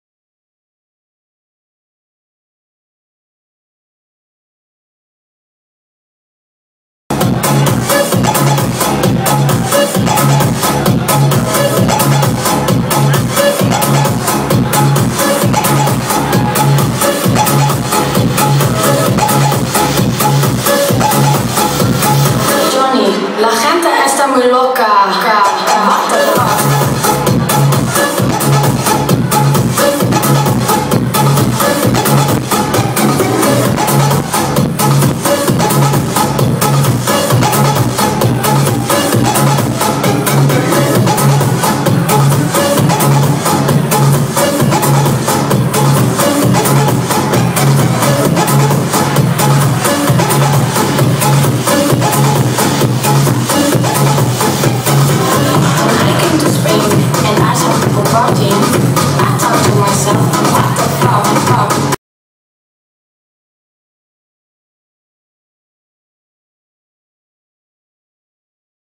Two Sexy girls Halloween Insomnia Walking Street Pattaya Thailand
Bangkokbargirl.com presents Two Sexy girls Halloween Insomnia Walking Street Pattaya Thailand.